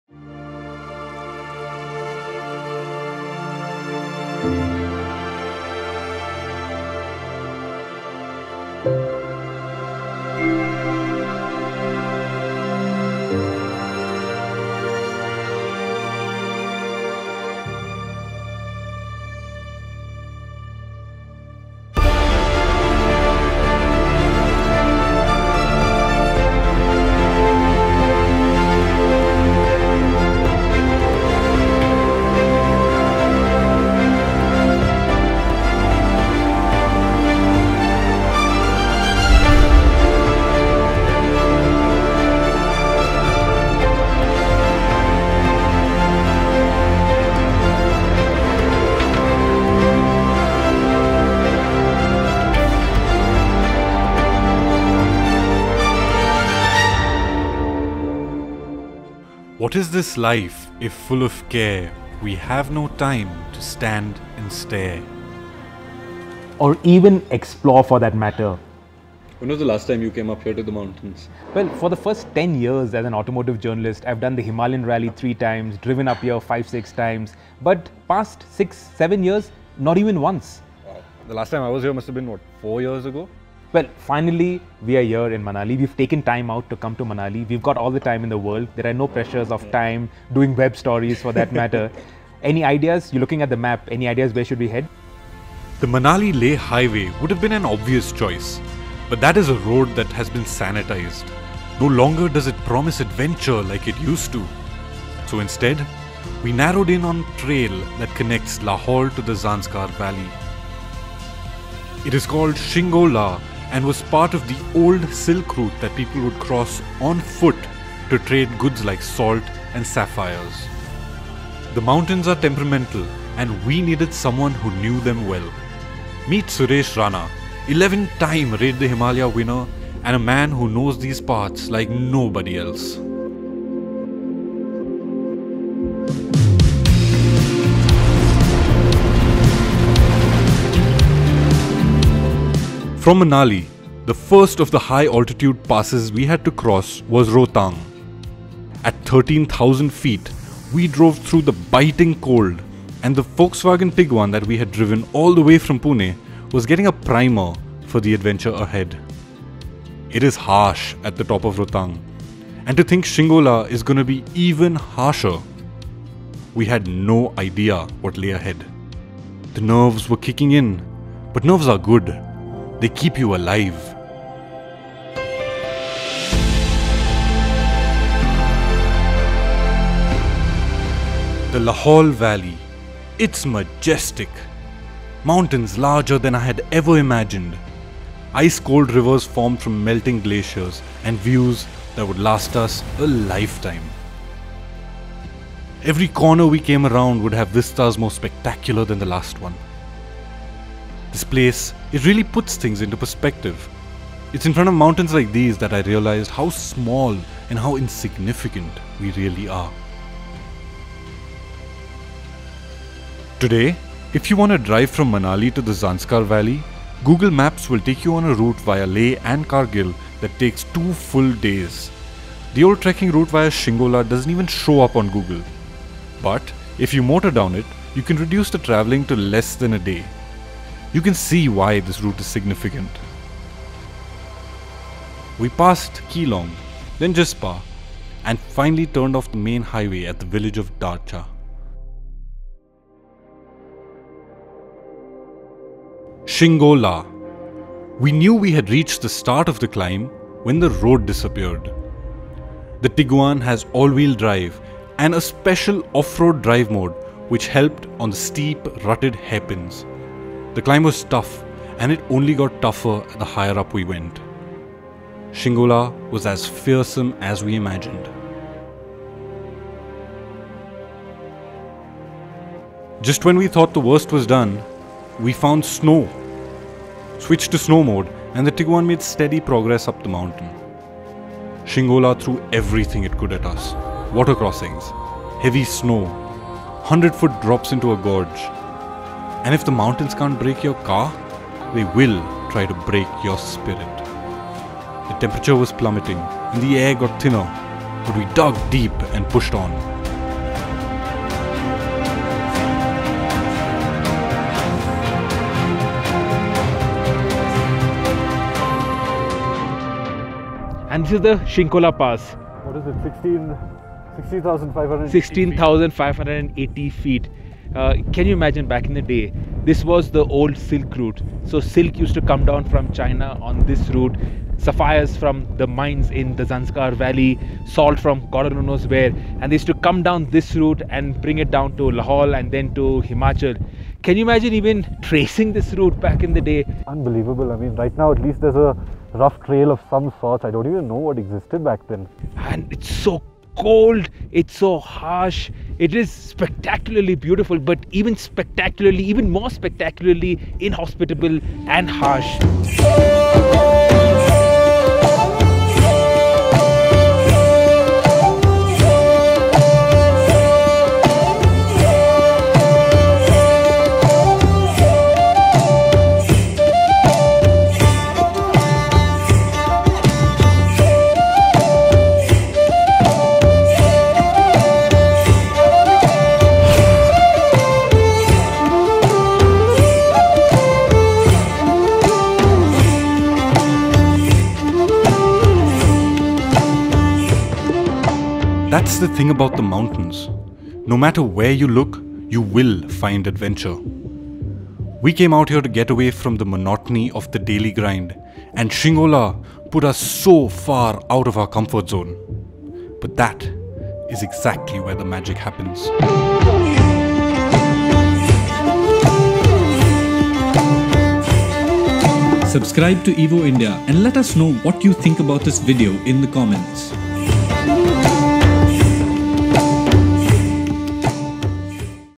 Volkswagen Tiguan SUV climbs to 16500ft in the Himalayas | evo India
In the Himalayas at 16,500 ft, the route which connects the Lahaul Valley to Zanskar in Ladakh gets treacherous. So what better reason than to put the new Tiguan's all-wheel drive system to work. Watch this video to see how we conquered the Shingo La pass with the Volkswagen Tiguan.